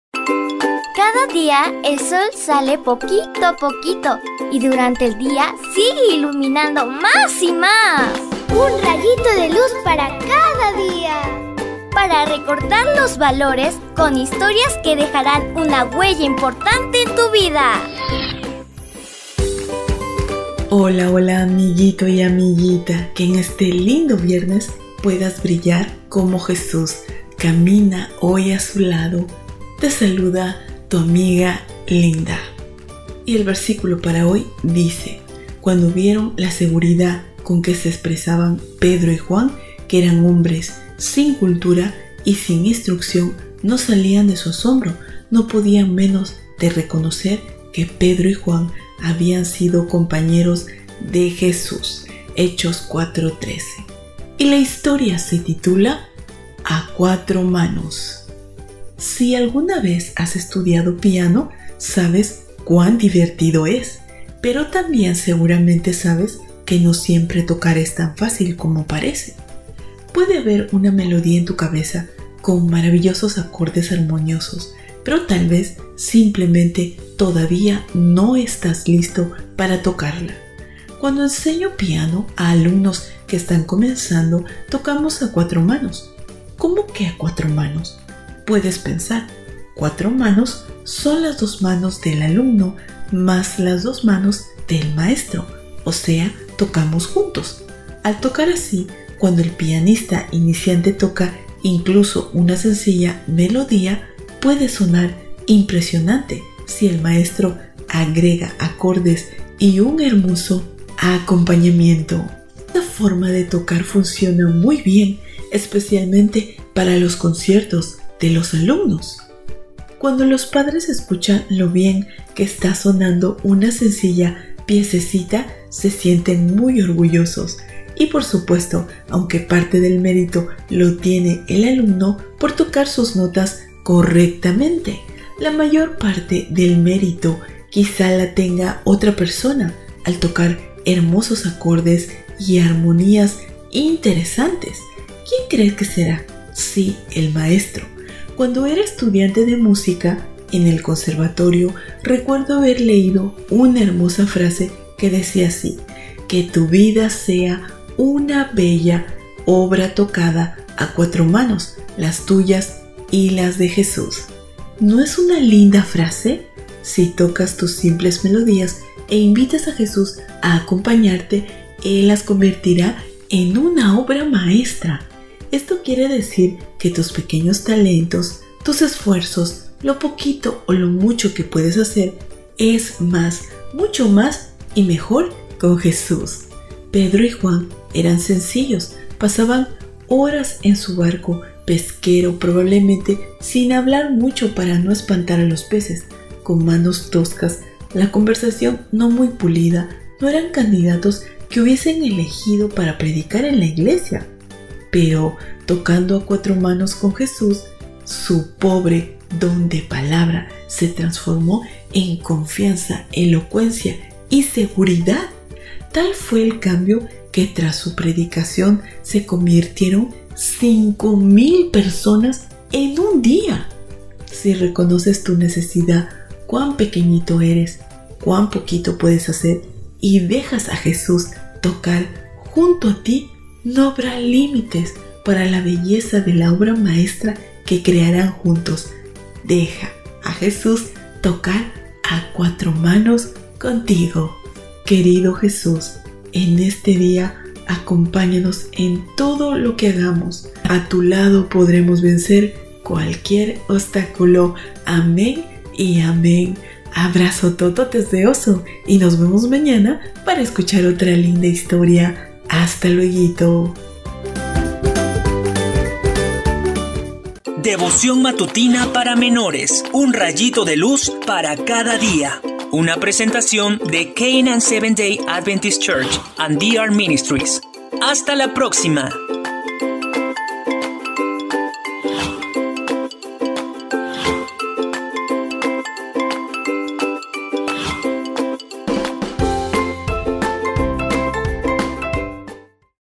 Devoción Matutina Para Menores - 07 de Octubre del 2022
07 DE OCTUBRE
A CUATRO MANOS
"CUANDO VIERON LA SEGURIDAD CON QUE SE EXPRESABAN PEDRO Y JUAN, QUE ERAN HOMBRES SIN CULTURA Y SIN INSTRUCCIÓN, NO SALÍAN DE SU ASOMBRO. [...] NO PODÍAN MENOS DE RECONOCER QUE PEDRO Y JUAN HABÍAN SIDO COMPAÑEROS DE JESÚS" (HECHOS 4:13, BLP).
Si alguna vez has estudiado piano, ¡sabes cuán divertido es! Pero también seguramente sabes que no siempre tocar es tan fácil como parece. Puede haber una melodía en tu cabeza, con maravillosos acordes armoniosos, pero tal vez simplemente todavía no estás listo para tocarla.
Cuando enseño piano a alumnos que están comenzando, tocamos a cuatro manos. "¿Cómo a cuatro manos?", puedes pensar. Cuatro manos son las dos manos del alumno más las dos manos del maestro. O sea, tocamos juntos. Al tocar así, cuando el pianista iniciante toca incluso una sencilla melodía, puede sonar impresionante si el maestro agrega acordes y un hermoso acompañamiento. Esta forma de tocar funciona muy bien, especialmente para los conciertos de alumnos. Cuando los padres escuchan lo bien que está sonando una sencilla piecita, se sienten muy orgullosos. Y, por supuesto, aunque parte del mérito lo tiene el alumno por tocar sus notas correctamente, la mayor parte del mérito quizá la tenga otra persona, al tocar hermosos acordes y armonías interesantes. ¿Quién crees que será? Sí, el maestro.
Cuando era estudiante de música en el Conservatorio, recuerdo haber leído una hermosa frase, que decía así: “Que tu vida sea una bella obra tocada a cuatro manos: las tuyas y las de Jesús”. ¿No es una linda frase? Si tocas tus simples melodías e invitas a Jesús a acompañarte, ¡él las convertirá en una obra maestra! Esto quiere decir que tus pequeños talentos, tus esfuerzos, lo poquito o lo mucho que puedas hacer, es más, mucho más y mejor con Jesús.
Pedro y Juan eran sencillos. Pasaban horas en su barco pesquero, probablemente sin hablar mucho para no espantar a los peces. Con manos toscas, la conversación no muy pulida, no eran candidatos que hubieras elegido para predicar en la iglesia. Pero “tocando a cuatro manos” con Jesús, su pobre don de palabra se transformó en confianza, elocuencia y seguridad. Tal fue el cambio que tras su predicación se convirtieron 5.000 personas en un día. Si reconoces tu necesidad, cuán pequeñito eres, cuán poquito puedes hacer y dejas a Jesús “tocar” junto a ti, no habrá límites para la belleza de la obra maestra que crearán juntos. Deja a Jesús tocar a cuatro manos contigo.
Cinthya

UN RAYITO DE LUZ PARA CADA DIA